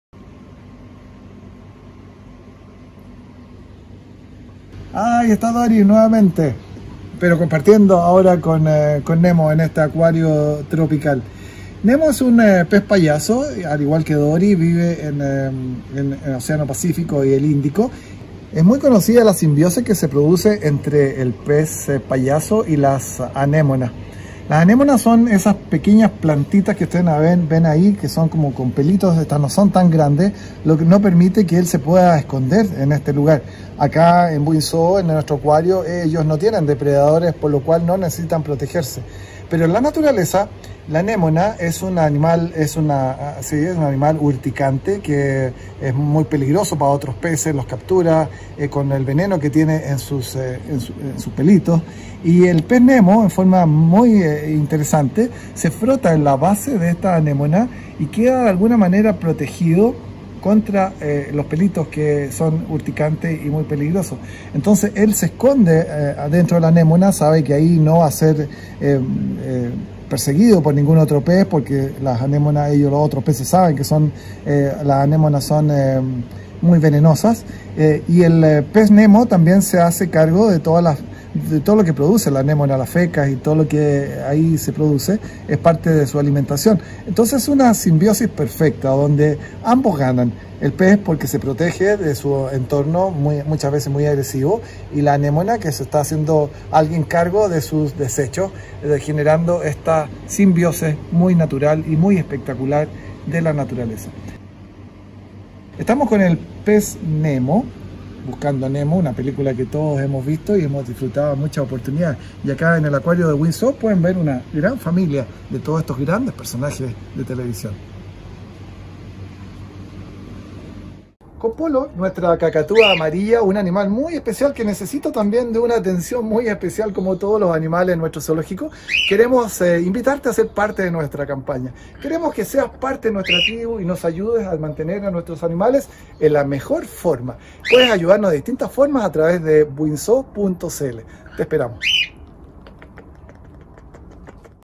BUIN ZOO TE ACOMPAÑA EN CASA (Buscando a Nemo)
Hoy seguimos buceando por nuestro acuario junto al Dr. Zoo para encontrar a Nemo, el famoso pez payaso que acompaña a Dori en sus aventuras. No te pierdas este increíble capítulo de BuinZooTeAcompañaEnCasa

Si apoyas nuestra labor, ingresa a y conoce las alternativas para ayudarnos a seguir trabajando por el cuidado de nuestros animales.